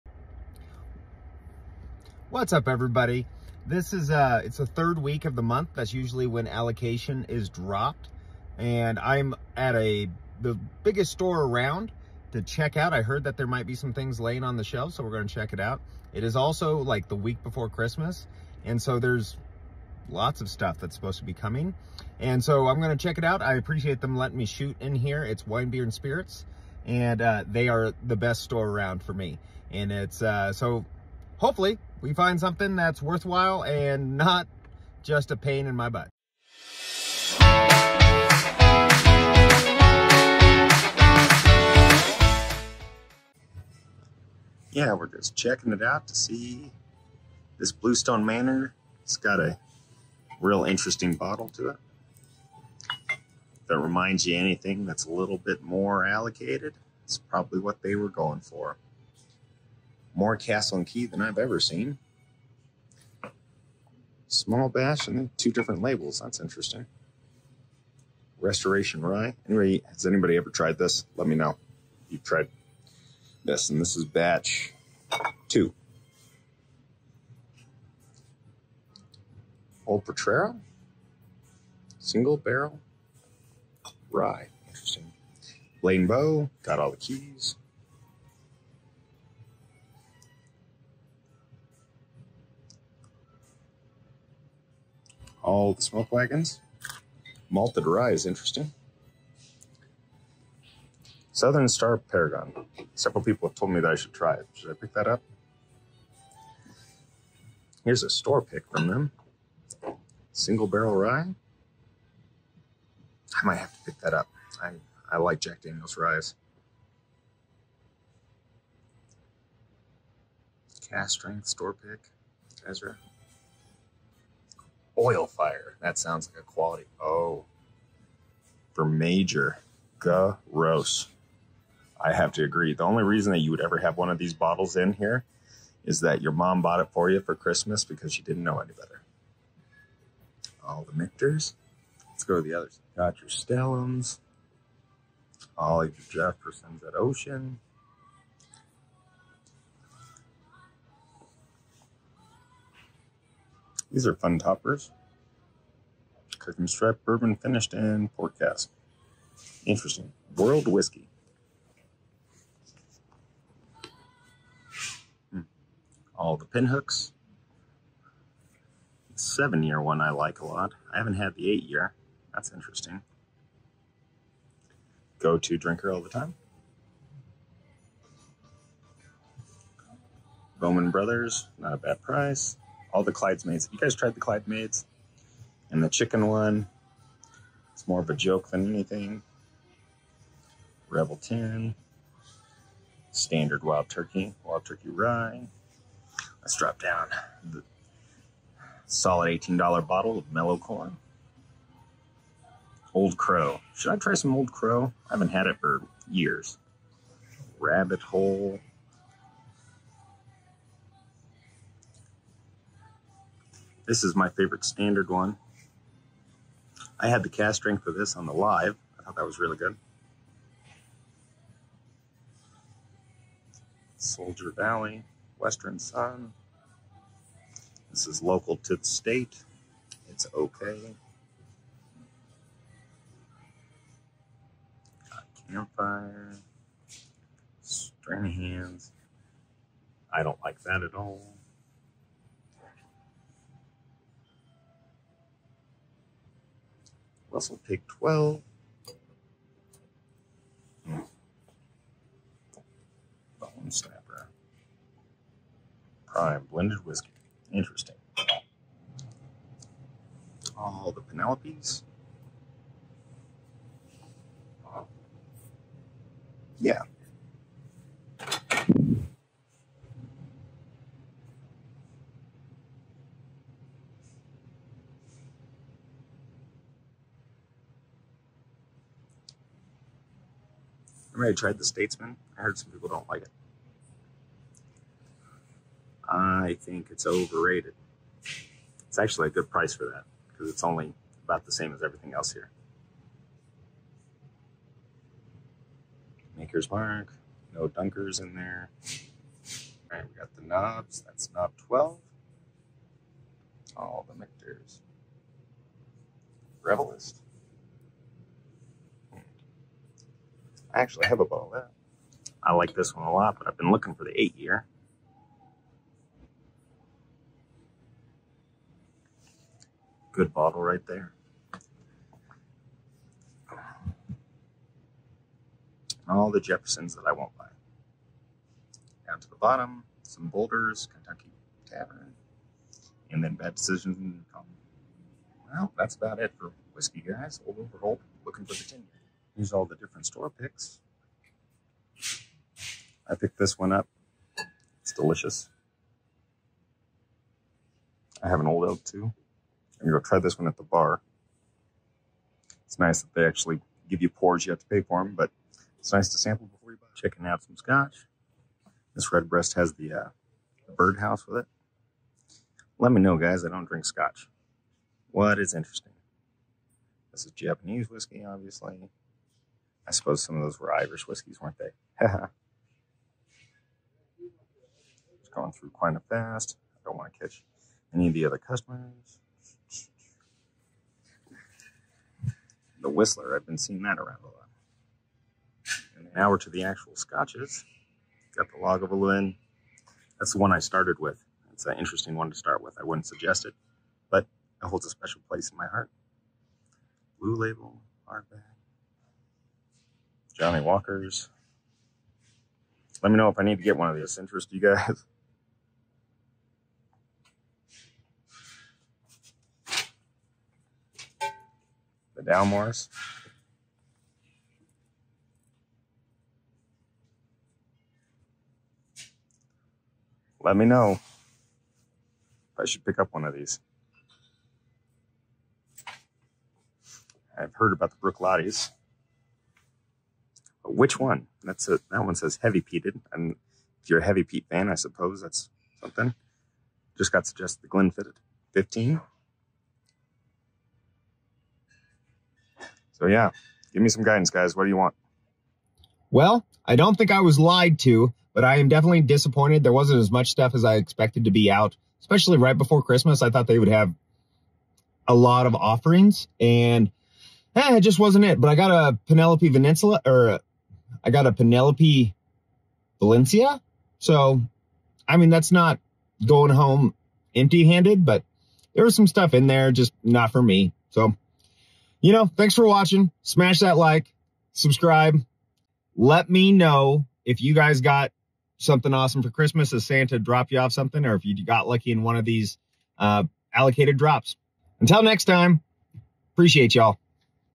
Bourbon Hunting at the best store in my area!
Looking for those special bottles that are supposed to be dropping soon.  Checking out the best/biggest store in my area on allocation drop day!.